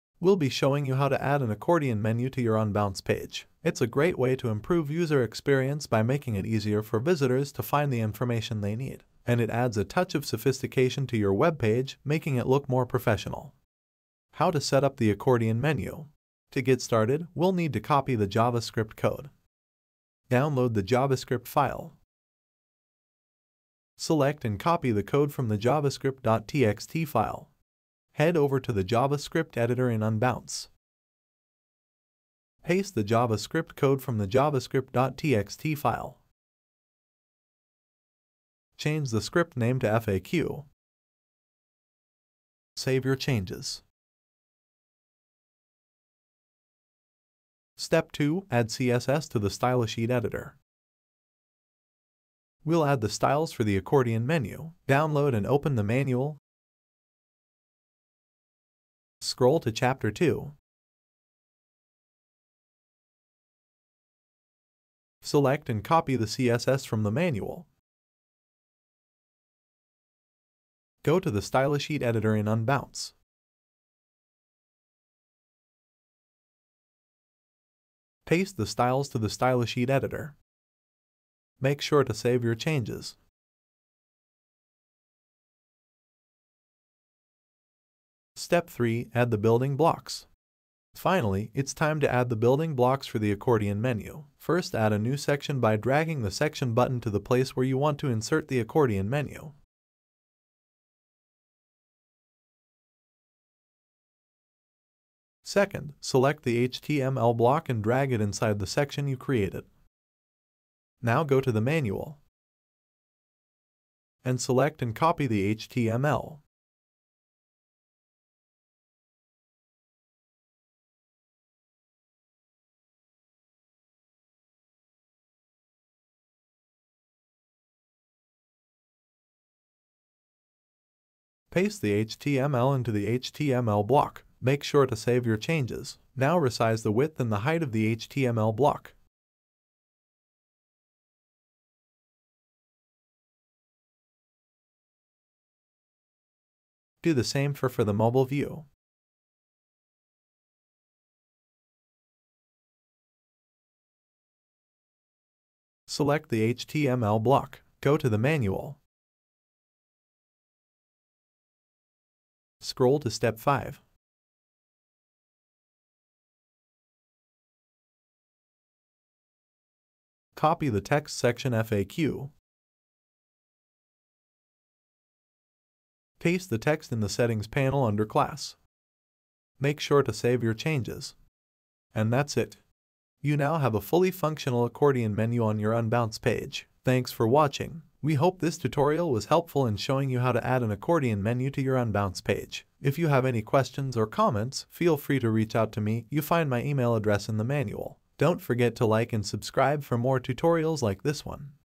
Unbounce Accordion Menu
In this step-by-step tutorial, we'll show you how to add a professional and sophisticated accordion menu to your Unbounce page. Say goodbye to cluttered pages and hello to an improved user experience. Watch now to learn how to add an accordion menu to your Unbounce page in just a few simple steps.